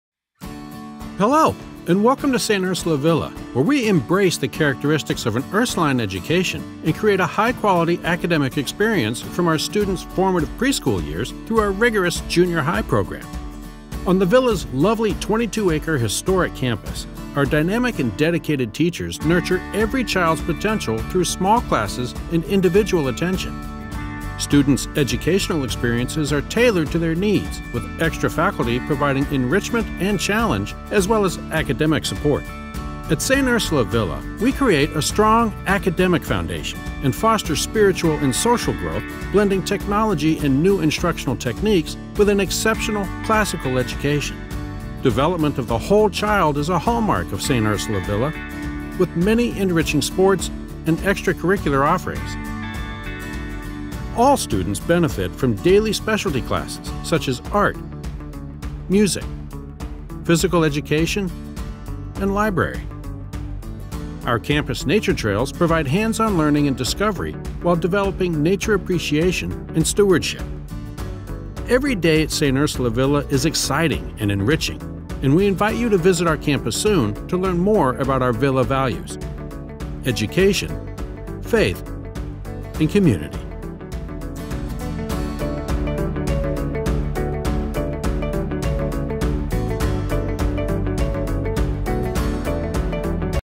Discover St. Ursula Villa and the Villa Values: Education, Faith, Community
Learn more about St. Ursula Villa's outstanding education for Toddlers through Junior High.